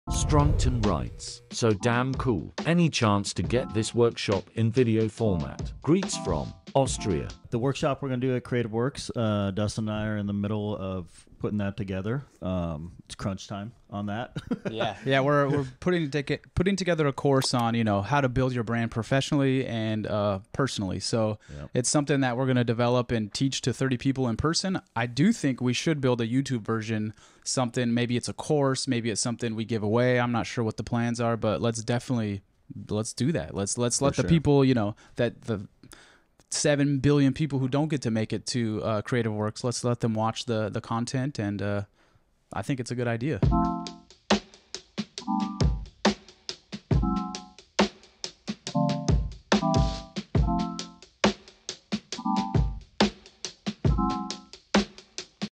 Will your Creative Works course on Personal and Professional Branding be RECORDED?
Stroncton writes, "So damn cool! Any chance to get this workshop [Creative Works] in video format? Greetings from Austria"

Thanks for the question about this course! It's getting down to crunch time and Dan and Dustin are putting finishing touches on the course now. We probably will make the content available in some form for video on our YouTube channel after the presentation, so stay tuned to our channel here and Instagram for more info!

In the meantime, we'd love to see you in person if you can make it! Registration link is below: